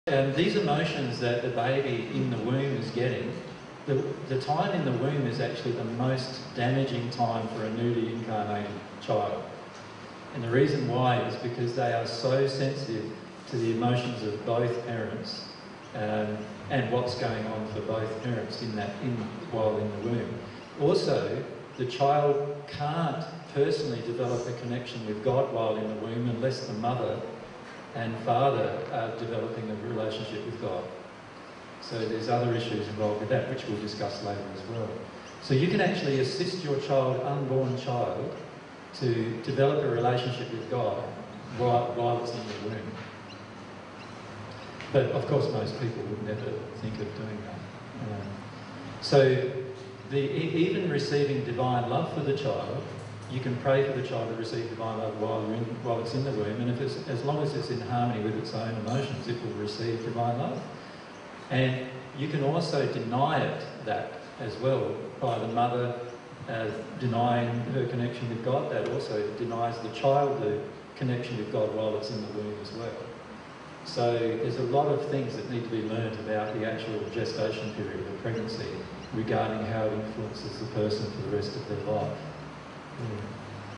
Jesus on Unborn Children Receiving God's Love in the Womb
Jesus explains how parents can help their children to receive God's love while they are in the womb.

This is a section of the presentation by Jesus entitled "20090307 Human Relationships - Parenting Children S1P1".

dtstart:002538:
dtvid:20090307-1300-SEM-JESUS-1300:
dtmd5:5a76f1ff9d43173e2f4caa59848cccfc: